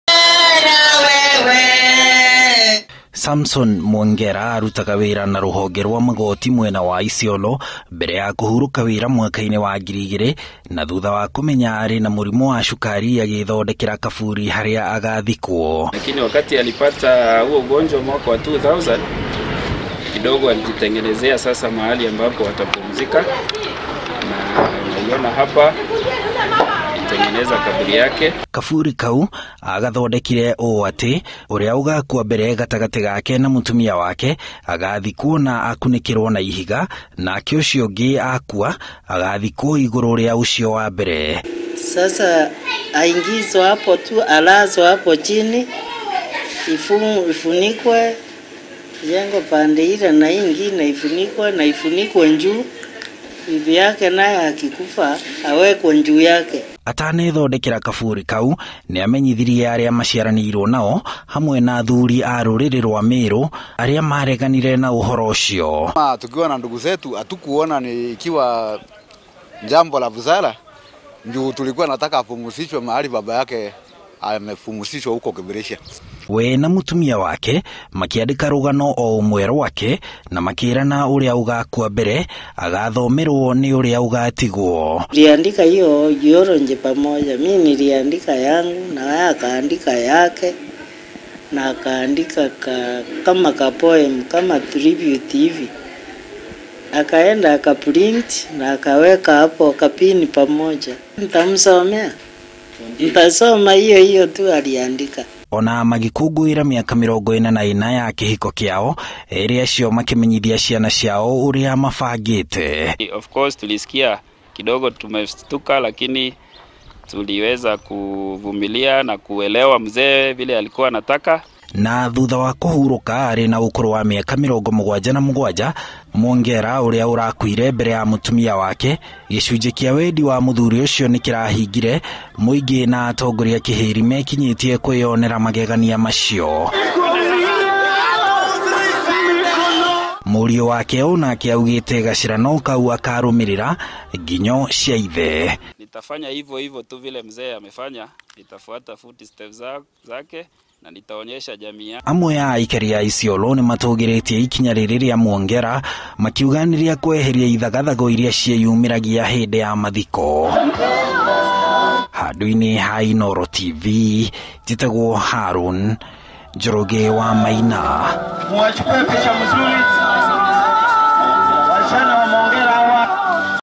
Wendi wa mutigairi gukinyanirio, Isiolo
Kurari na igongona ria mwanya ria mathiko ma muonjoria umwe mwena wa Milimani kaunti-ini ya Isiolo, uria miaka 19 mihituku ethondekeire kaburi gake, na akiuga wee na mutumia wake magathikwo hau, uria ugakua mbere athikwo rungu rwa ucio ungi. Samson Mwongera, mari na mutumia wake ningi nimeyandikiire rugano, na makiirana uria ugakua mbere agathomerwo ni uria ugatigwo, maundu maria mamenyithirie ciana ciao, onagutuika ni mugaru na unduire wa Ameru.